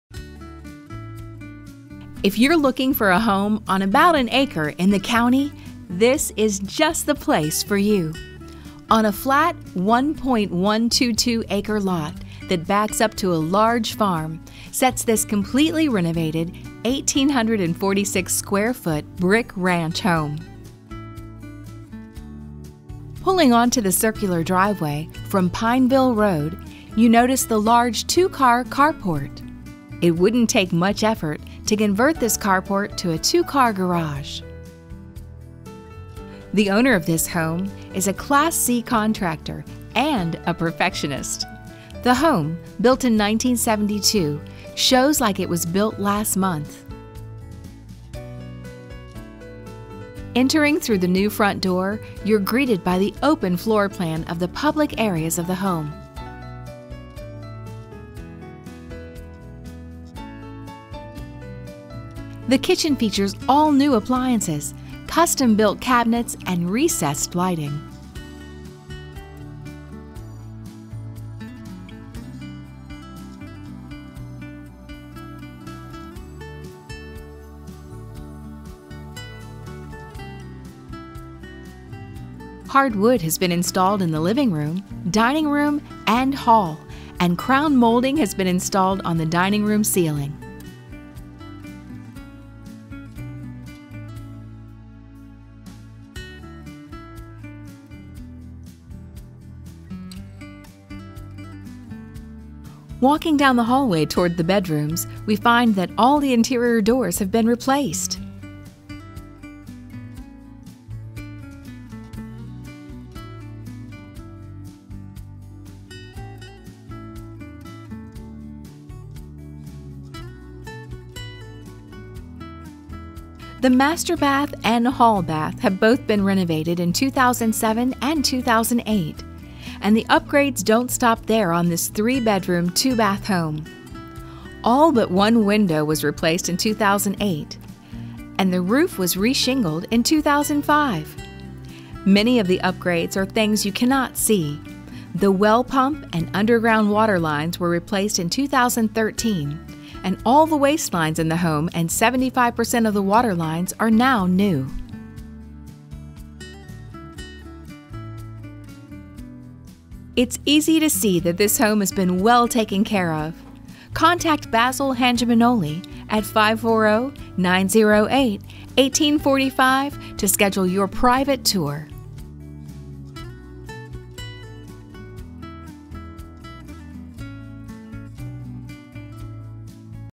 4001 Pineville Road, Port Republic MLS#538826
All brick ranch with major renovations;  windows, doors, all new appliances (2015), custom built cabinets, hardwood in dining room, new well pump, indoor supplemental wood furnace and more!